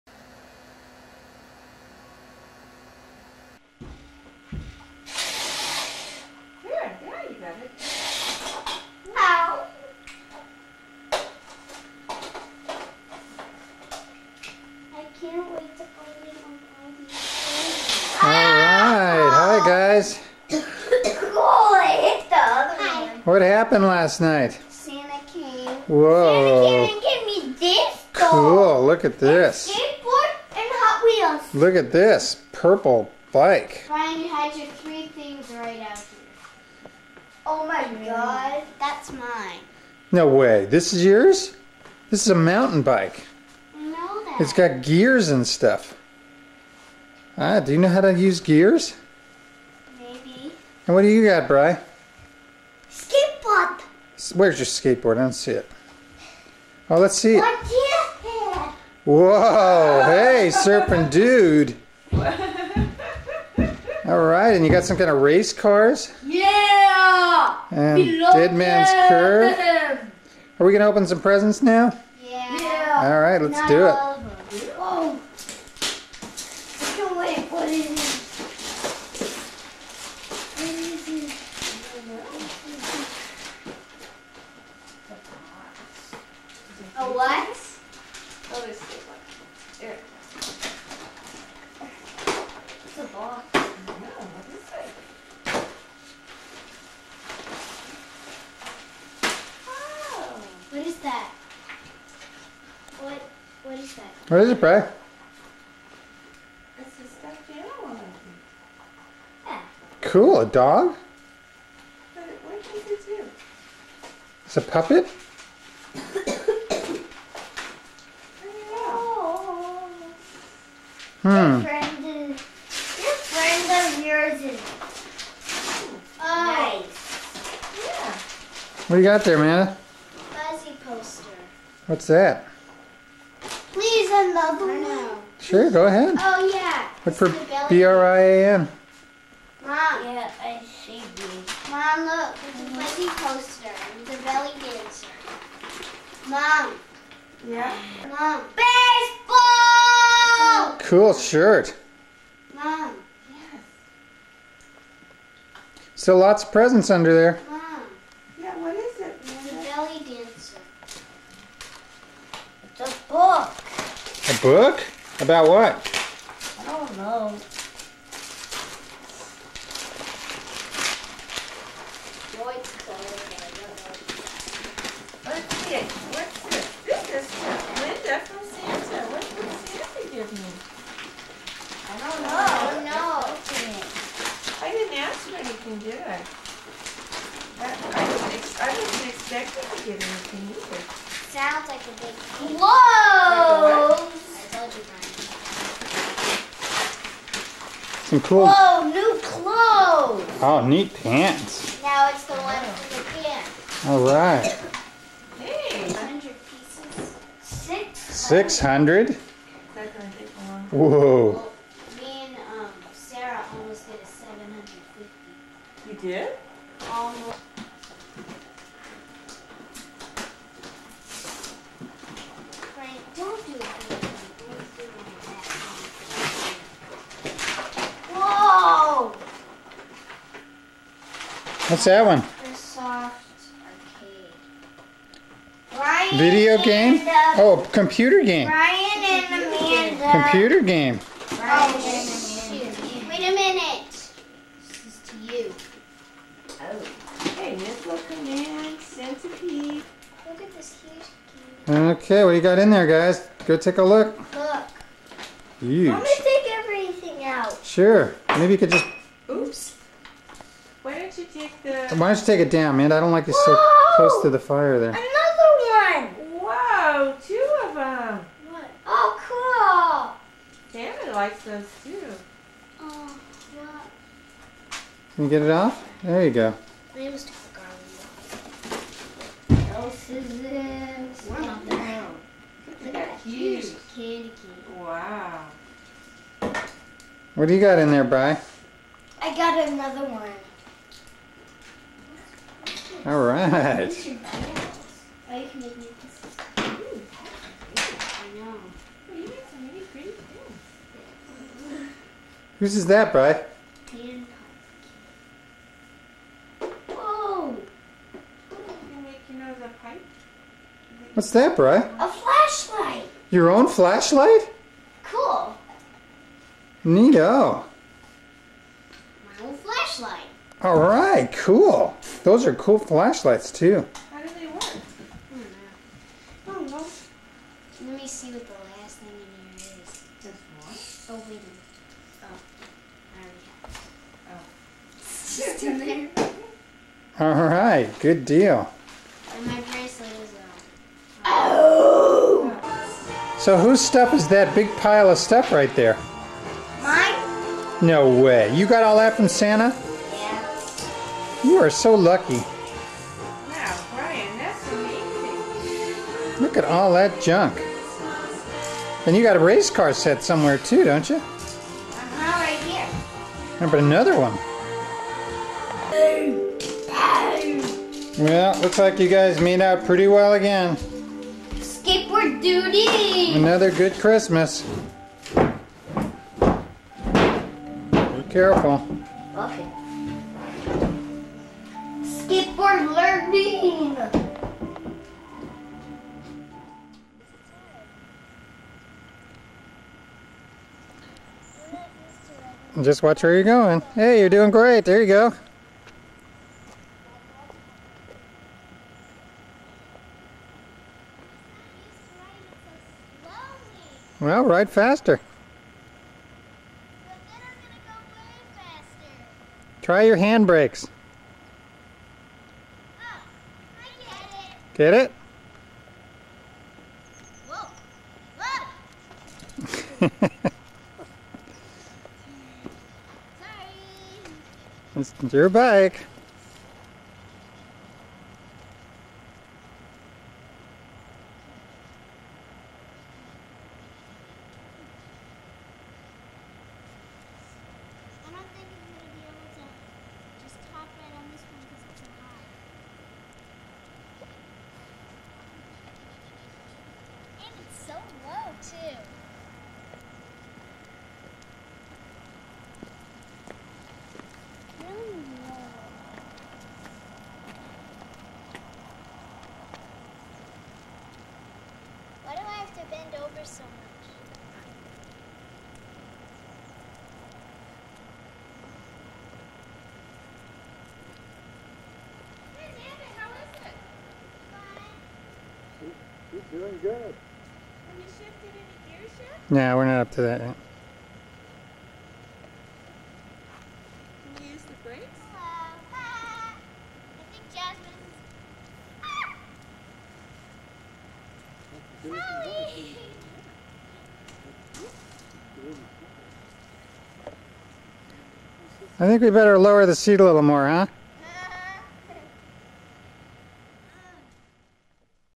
Chirstmas at Lamson-Cashion San Carlos home 12/25/1995
Lamson-Cashion family on Christmas morning at Kelton Ave home in San Carlos, CA. Amanda riding her new bike.